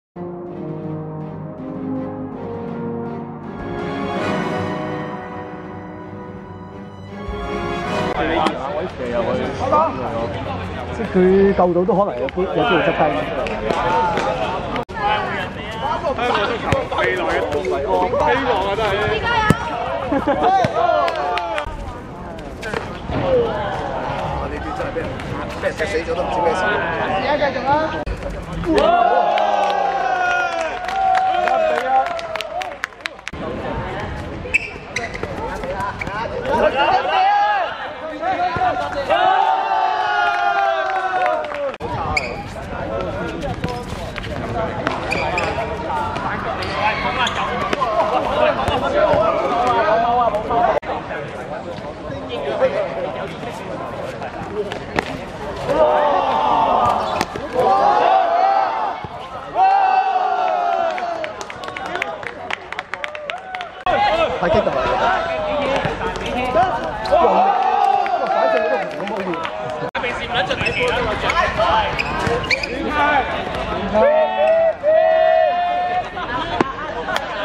藍鳥12人vs獨角獸(2016.2.10.赤口盃分組賽)精華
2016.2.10.油塘四人塲 12:48 藍鳥12人(藍) 4 : 1 獨角獸(白)